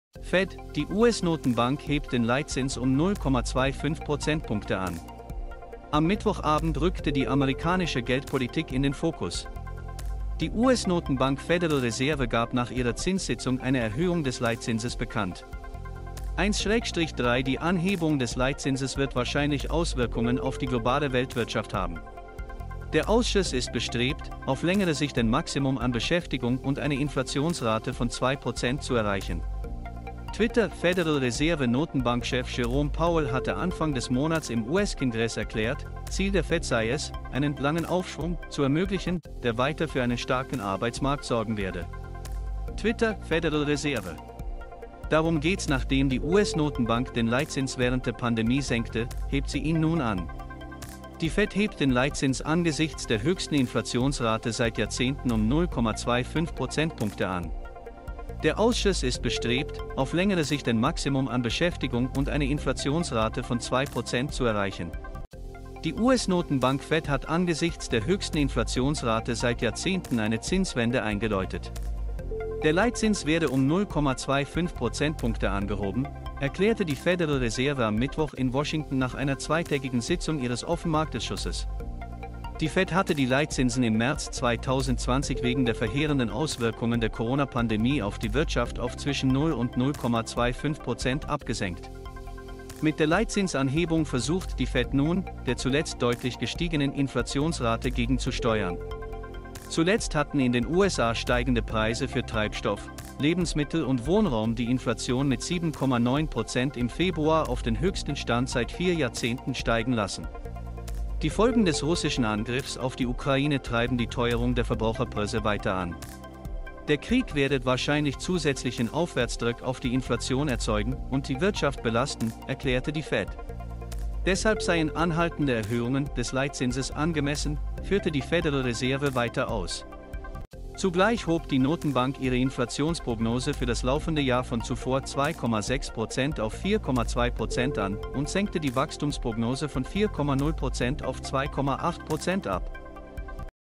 Federal Reserve: Die Fed wird die Zinsen um 0,25 Prozentpunkte anheben
Die US-Geldpolitik stand am späten Mittwoch im Fokus. Die Fed kündigte nach der Zinssitzung eine Zinserhöhung an.
Fed: Die Fed erhöht die Zinsen um 0,25 Prozentpunkte

Eine Zinserhöhung um 1/3 könnte Auswirkungen auf die Weltwirtschaft haben. Ziel des Komitees ist es, langfristig maximale Beschäftigung und 2 Prozent Inflation zu erreichen. Twitter/Federal Reserve Der Vorsitzende der Federal Reserve, Jerome Powell, sagte Anfang dieses Monats vor dem US-Kongress, dass die Zentralbank ein „langfristiges Aufwärtspotenzial“ anstrebe, das weiterhin einen starken Arbeitsmarkt sicherstellen werde. Twitter/Federal Reserve

Es geht darum, dass die Fed die Zinsen jetzt anhebt, nachdem sie sie während der Pandemie gesenkt hat.

Die Fed erhöht die Zinsen um 0,25 Prozentpunkte inmitten der höchsten Inflation seit Jahrzehnten.

Ziel des Komitees ist es, langfristig maximale Beschäftigung und 2 Prozent Inflation zu erreichen.

Die Fed hat angesichts der höchsten Inflationsrate seit Jahrzehnten einen Zinsaufschwung signalisiert. Die Fed hatte am Mittwoch nach einer zweitägigen Sitzung ihres Offenmarktausschusses in Washington angekündigt, den Leitzins um 0,25 Prozentpunkte anzuheben. Die Federal Reserve senkte die Zinssätze im März 2020 aufgrund der verheerenden Auswirkungen der Coronavirus-Pandemie auf die Wirtschaft auf null bis 0,25 %.

Mit der Zinserhöhung versucht die Fed nun, die zuletzt stark gestiegene Inflation auszugleichen. In jüngerer Zeit haben steigende US-Treibstoff-, Lebensmittel- und Immobilienpreise die Inflation im Februar auf ein 40-Jahres-Hoch von 7,9 % getrieben. Die Folgen des russischen Angriffs auf die Ukraine treiben die Verbraucherpreise weiter in die Höhe.

Die Fed sagte, der Krieg „könnte zusätzlichen Aufwärtsdruck auf die Inflation ausüben“ und die Wirtschaft belasten. Deshalb seien „dauerhafte Erhöhungen“ der Leitzinsen „angemessen“, so die Fed weiter. Unterdessen erhöhte die Zentralbank ihre Inflationsprognose für dieses Jahr von 2,6 % auf 4,2 % und senkte ihre Wachstumsprognose von 4,0 % auf 2,8 %.